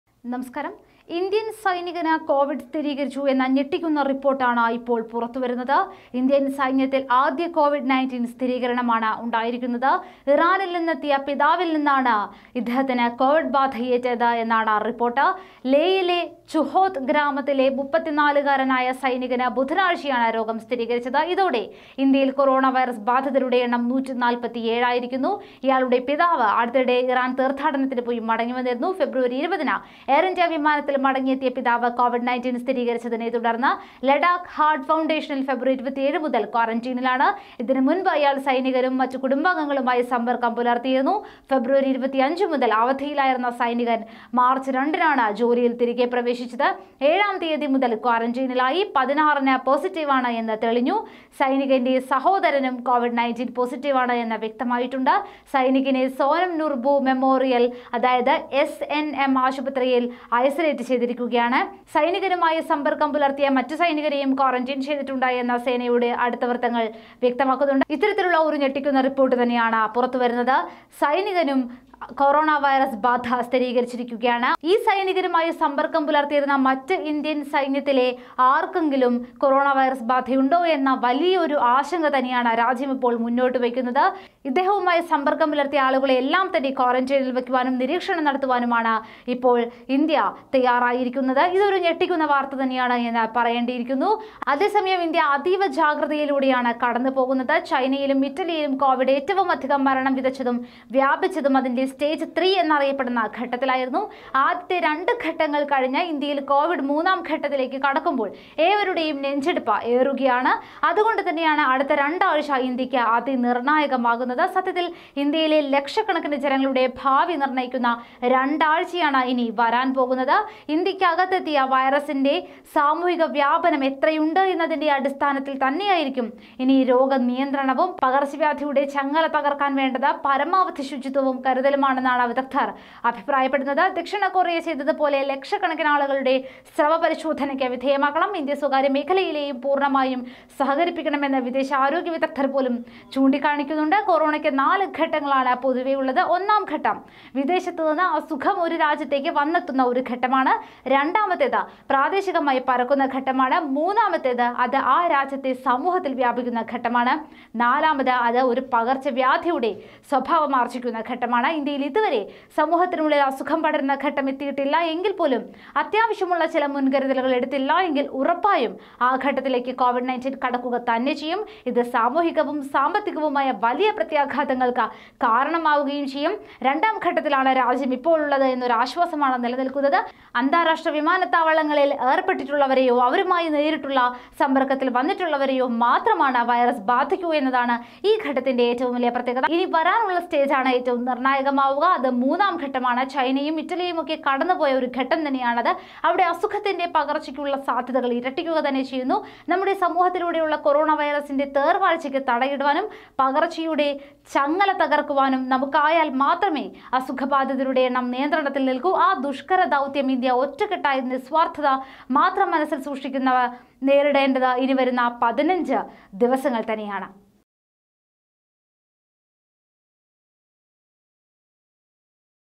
34കാരനായ ഇന്ത്യന്‍ സൈനികന് കോവിഡ് 19 സ്ഥിരീകരിച്ചു.. I India
ഇന്ത്യൻ സൈന്യത്തിൽ ആദ്യ കോവിഡ്–19 സ്ഥിരീകരിച്ചു. ലേയിലെ ഛുഹോത്ത് ഗ്രാമത്തിലെ 34കാരനായ സൈനികന് ബുധനാഴ്ചയാണ് രോഗം സ്ഥിരീകരിച്ചത്. ഇതോടെ ഇന്ത്യയിൽ കൊറോണ വൈറസ് ബാധിച്ചവരുടെ എണ്ണം 147 ആയി.